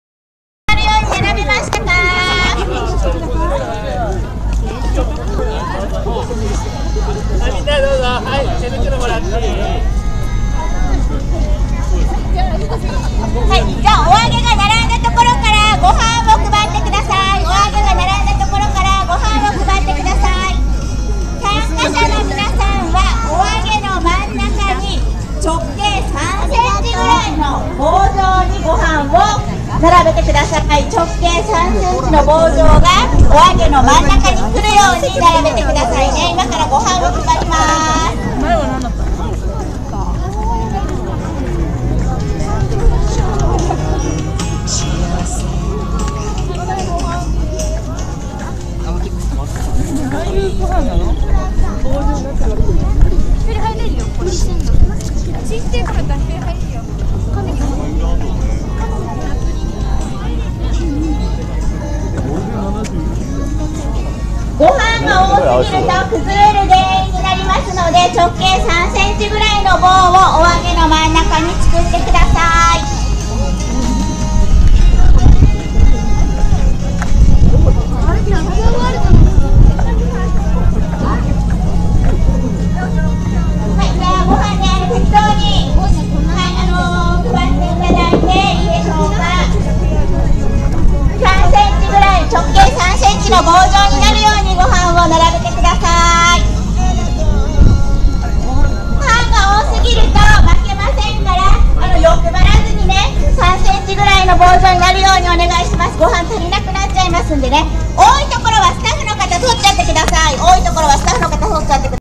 イバラッキーが笠間いきなりいなり寿司を応援
71メートルのいなり寿司を250人以上で作る所です